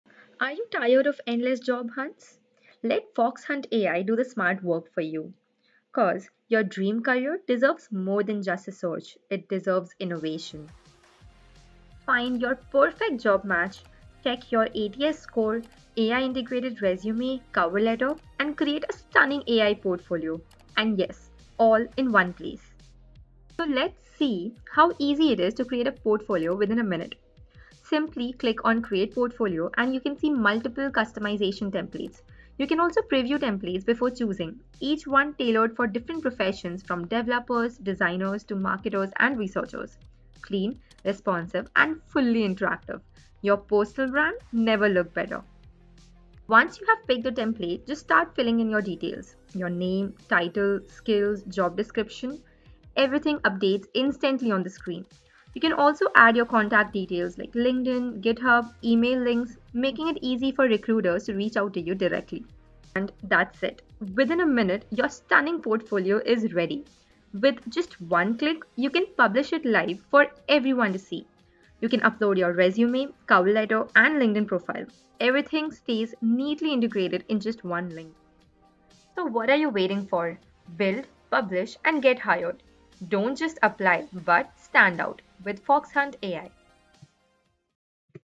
Build a Job-Ready Portfolio in Minutes (Track Views + Contacts) | portfolios.studio
In today’s market, a portfolio isn’t optional, it’s how you stand out.

Meet portfolios.studio : go from “no site” to job-ready portfolio in minutes. No code. Clean, professional templates. And the best part? You can track interactions (visits, clicks) and manage every contact/lead in one place, built right in.

00:00 Why a portfolio matters now
00:22 Build a portfolio in minutes (no code)
00:55 Track views & clicks with built-in analytics
01:18 Capture and manage contacts in one place
01:45 Who this is for (Dev, Data, Design, PM, New Grad)
02:05 How to get started today

Why portfolios.studio

⚡️ Build fast: pick a template, paste your work, publish
📊 Analytics: see what hiring teams view and click
🤝 CRM: contact capture + simple lead management
🧩 Flexible sections for projects, case studies, and resumes

(Claim your URL, publish in minutes, and start tracking your traffic.)

Job seekers, career switchers, students/new grads, developers, data professionals, designers, product folks—anyone who needs a clean, trackable portfolio link on resumes, LinkedIn, and applications.

Work with us / feedback
Drop your portfolio link in the comments. I’ll review a few on video!

Chapters
00:00 Intro
00:22 Build
00:55 Track
01:18 Contacts
01:45 Who It’s For
02:05 Get Started